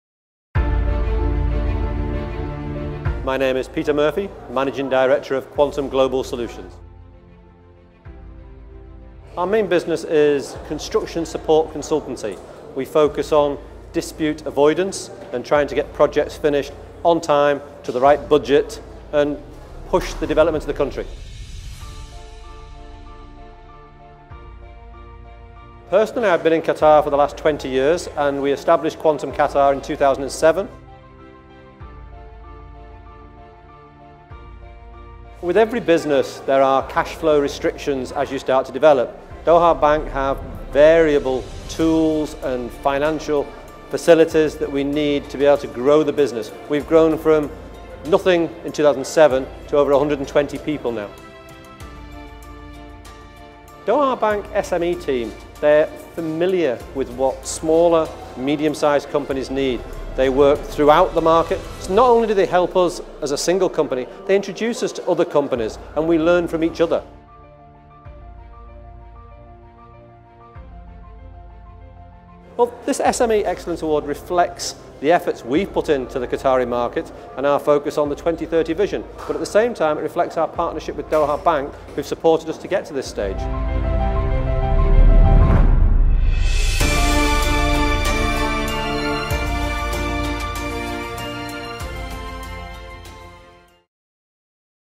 Doha Bank Six SME_Clients Among Top Performing SMEs in Qatar
We congratulate our customers for their excellent performance in Qatar market by forming viable business models and making an impact on the Qatar economy. Award winning customers are: New Era Controls, Olive International School, Q-Star Trading Co., Shahjan Trading & Contracting, Q Tickets& Events, Quantum Qatar.